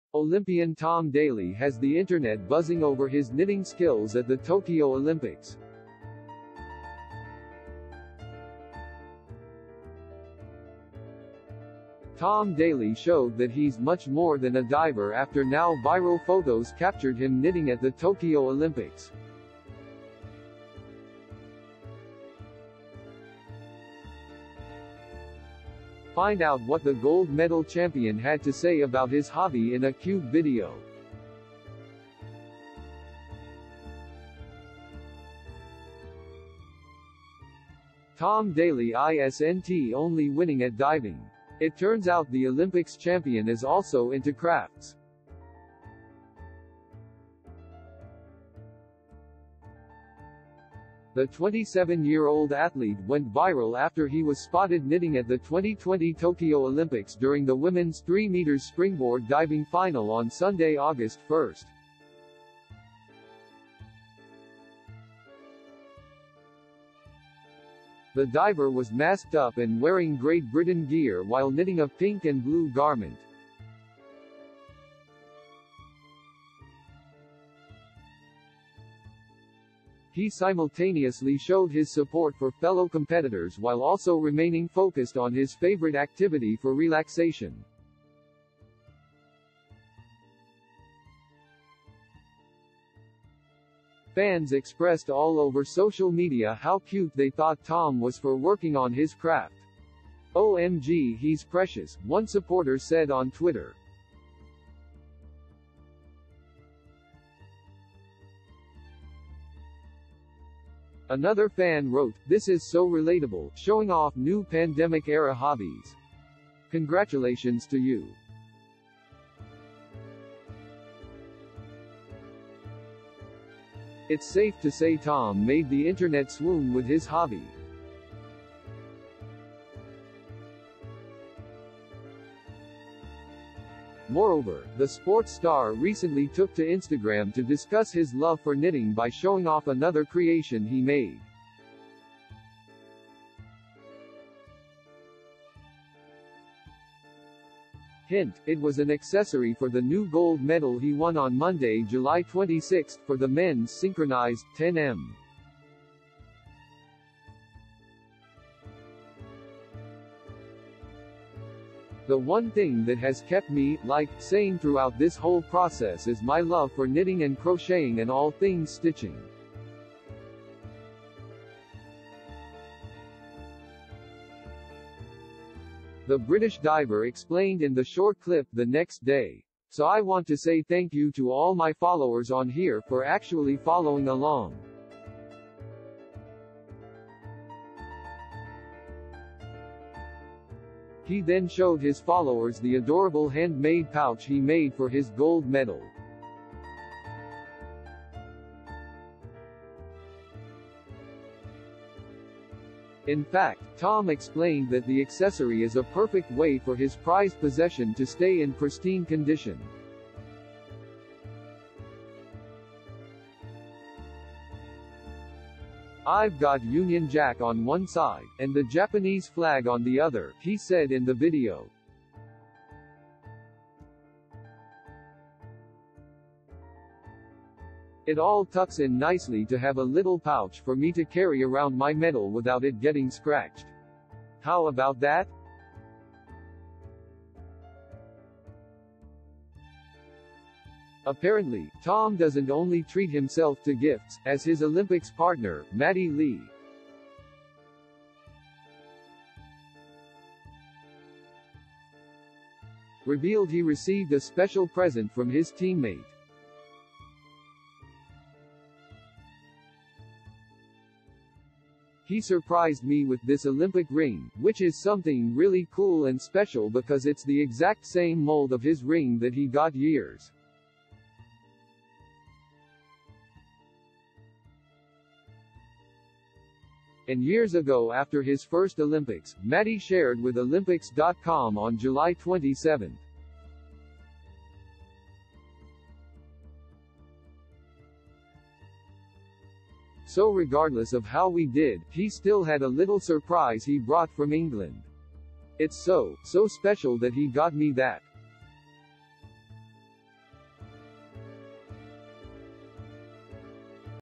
Olympian Tom Daley Has the Internet Buzzing Over His Knitting Skills at the Tokyo Olympics
Entertainment And Celebrity News With The Authority You Expect Everywhere You Are